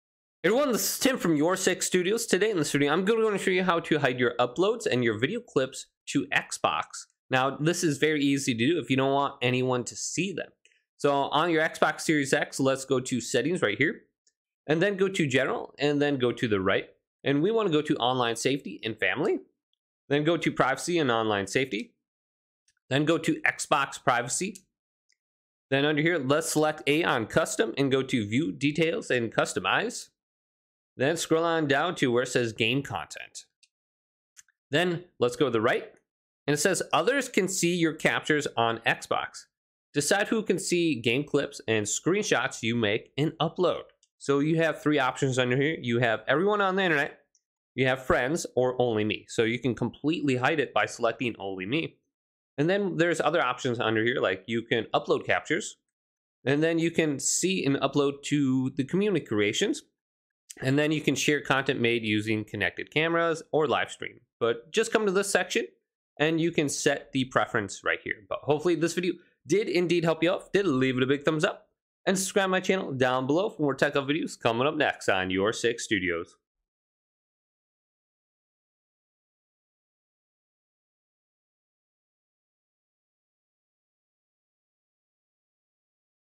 How to Hide your Uploads & Saved Video Clips on Xbox Series X (Don't Share Clips)
To hide your uploads and saved video clips on Xbox Series X, you can follow these steps:

Press the Xbox button on your controller to open the guide menu.
Scroll to the right to the Capture & Share page.
Select Capture Settings.
Press A on the Automatically Upload dropdown.
Select Don’t Upload.

This will disable the automatic uploading of your game clips and screenshots to the Xbox network or OneDrive